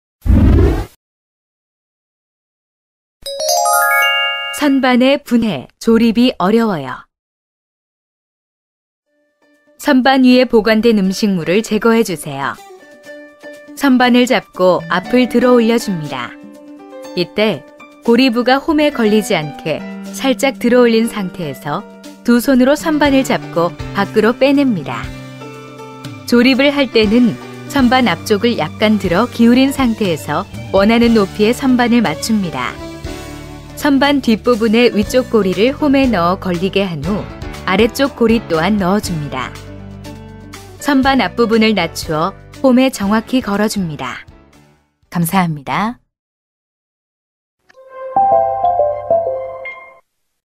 LG전자 냉장고 선반 분해조립이 어려워요
냉장고 선반 분해조립이 어려운 경우 조치방법입니다.